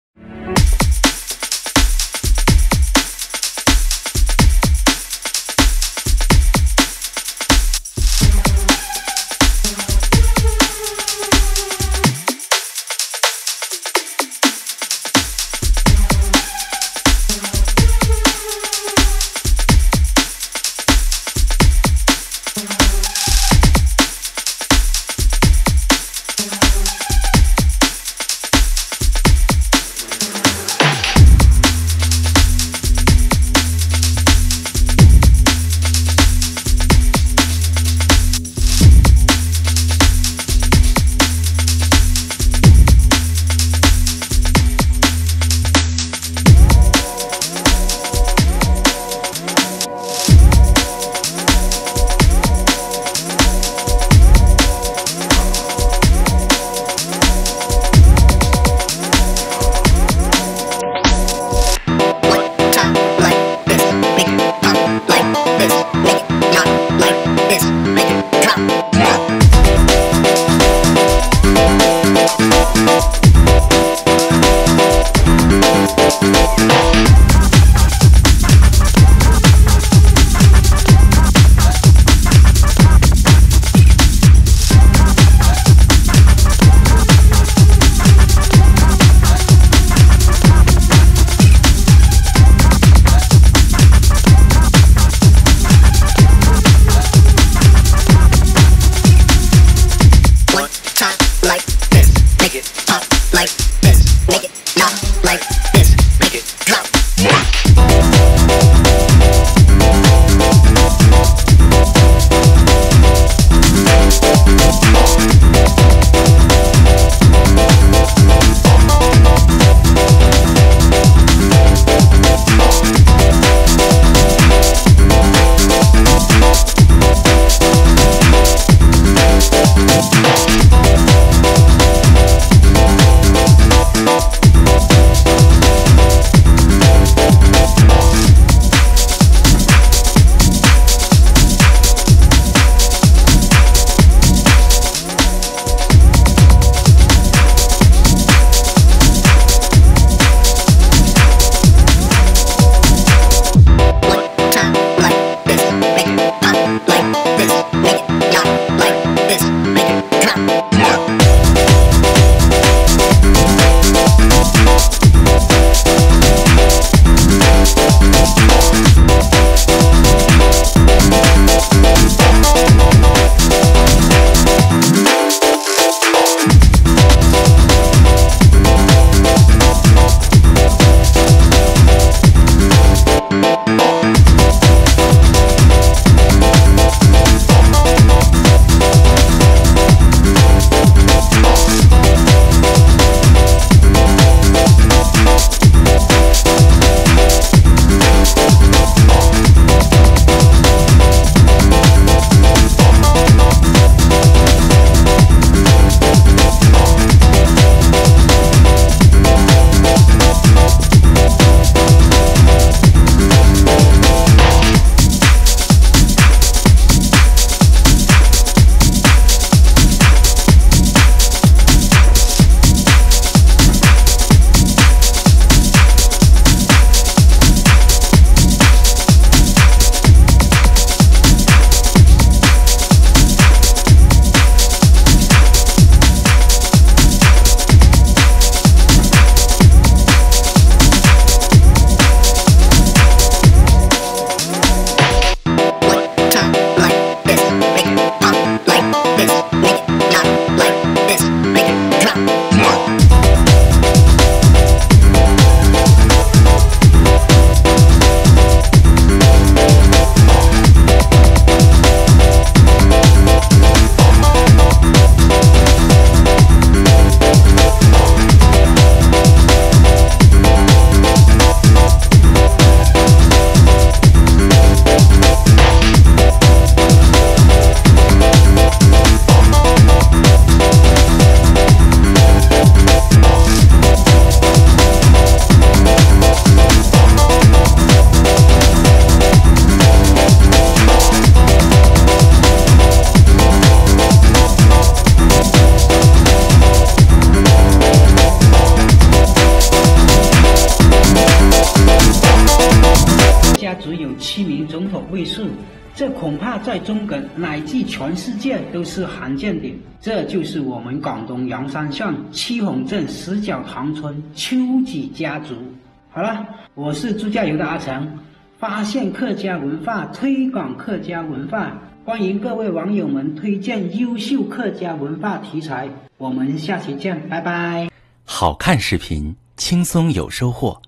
广东阳山丘氏七兄弟，枪林弹雨中护送孙中山先生，这个村真不简单【阿城视野】
大家好，我是阿城，我将用我的视角带你领略祖国的大好河山